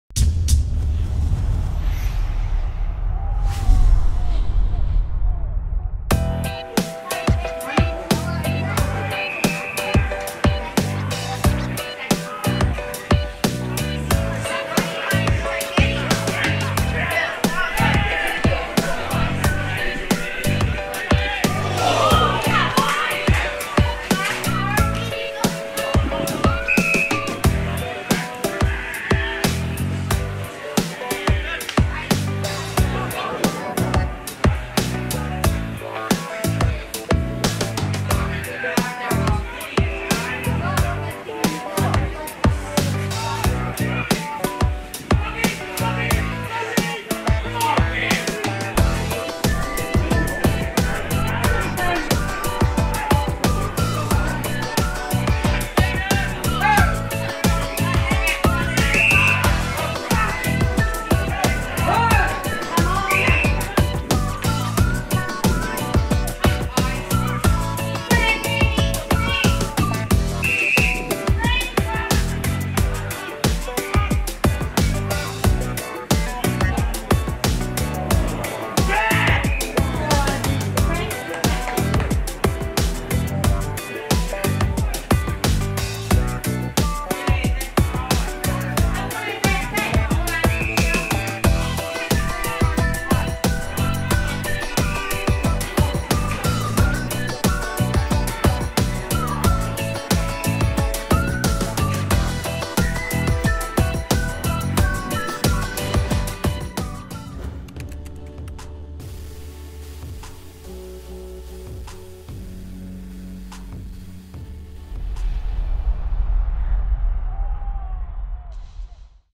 VFL Round 1, 2015 - Trent Dumont highlights
Highlights of Trent Dumont's game for North Ballarat in the VFL on Sunday.